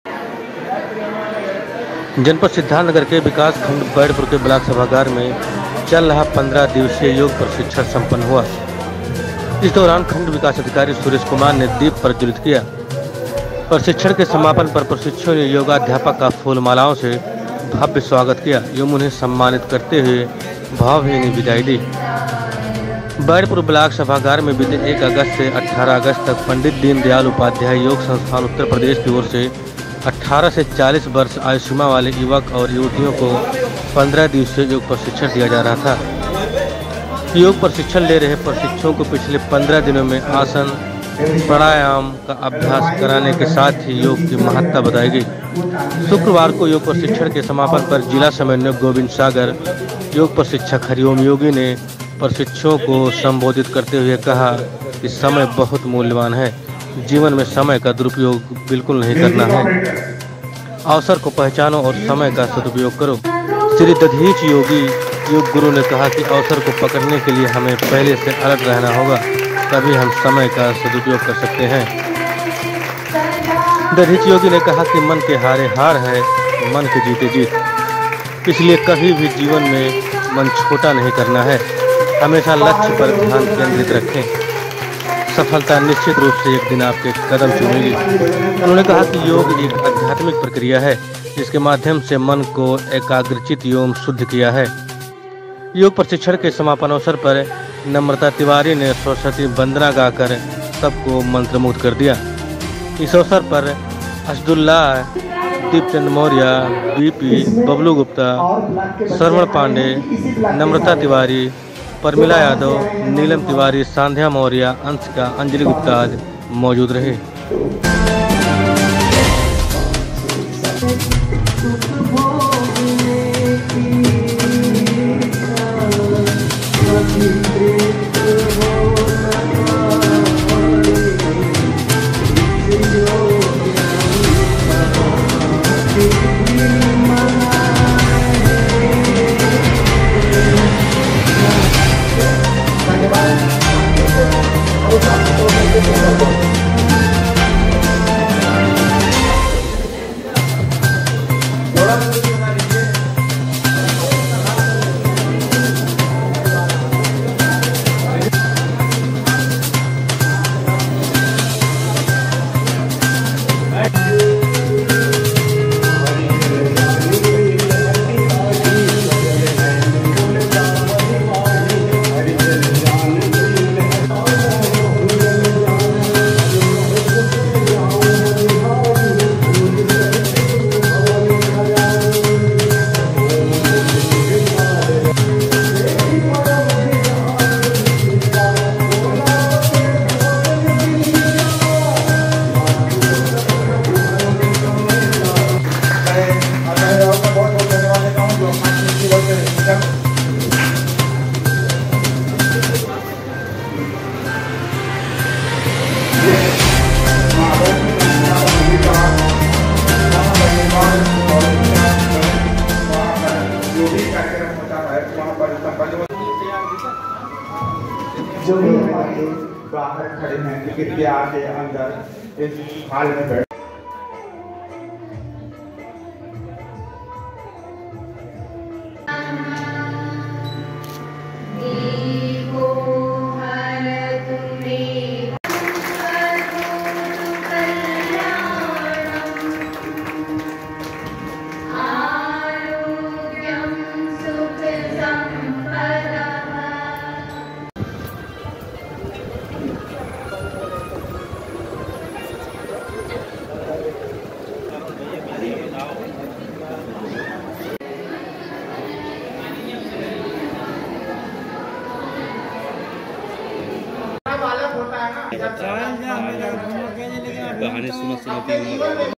SDDN: ब्लॉक सभागार में चल रहा 15 दिवसीय निःशुल्क योग प्रशिक्षण सम्पन्न।
कार्यक्रम के समापन पर दीप प्रज्वलित करते खंड विकास अधिकारी योग।

 प्रशिक्षण कार्यक्रम का समापन

बर्डपुर ब्लॉक सभागार में चल रहा 15 दिवसीय योग प्रशिक्षण सम्पन्न हुआ।
इस दौरान खंड विकास अधिकारी सुरेश कुमार ने दीप प्रज्वलित किया।
 प्रशिक्षण के समापन पर प्रशिक्षुओं ने योगा अध्यापक का फूल मालाओं से भव्य स्वागत किया एवं उन्हें  सम्मानित करते हुए भावभीनी विदाई दी।
 बर्डपुर ब्लाक सभागार में बीते 1 अगस्त 18 अगस्त तक पंडित दीनदयाल उपाध्याय योग संस्थान उत्तर प्रदेश की ओर से 18 से 40 वर्ष आयु सीमा वाले युवक और युवतियों को 15 दिवसीय योग प्रशिक्षण दिया जा रहा था।
योग प्रशिक्षण ले रहे प्रशिक्षुओं को पिछले 15 दिनों में आसान, प्राणायाम का अभ्यास कराने के साथ ही योग की महत्ता बताई गई। शुक्रवार को योग प्रशिक्षण के समापन पर जिला समन्वयक गोविंद सागर  योग प्रशिक्षक हरिओम योगी ने प्रशिक्षुओं को संबोधित करते हुए कहा कि समय बहुत मूल्यवान है, जीवन में समय का दुरुपयोग बिल्कुल नही करना है। अवसर को पहचानो और समय का सदुपयोग करो। श्री दधीच योगी (योग गुरु) ने कहा कि अवसर को पकड़ने के लिए हमें पहले से अलर्ट रहना होगा तभी हम समय का सदुपयोग कर सकते हैं।
दधीच योगी ने कहा कि मन के हारे हार है, मन के जीते जीत इसलिए कभी जीवन में मन छोटा नही करना है, हमेशा लक्ष्य पर ध्यान केंद्रित रखें सफलता निश्चित रूप से एक दिन कदम चूमेगी।
उन्होंने कहा कि योग एक आध्यात्मिक प्रक्रिया है जिसके माध्यम से मन को एकाग्रचित्त एवं शुद्ध किया है। योग प्रशिक्षण के समापन अवसर पर नम्रता तिवारी ने  सरस्वती वंदना  गाकर सबको मंत्रमुग्ध कर दिया।
इस अवसर पर असदुल्लाह,दीप चंद मौर्या, बीपी गुप्ता बबलू पांडेय,श्रवण दूबे,
नम्रता तिवारी, प्रमिला यादव, नीलम तिवारी, संध्या मौर्या, अंशिका, अंजली गुप्ता आदि मौजूद रहे।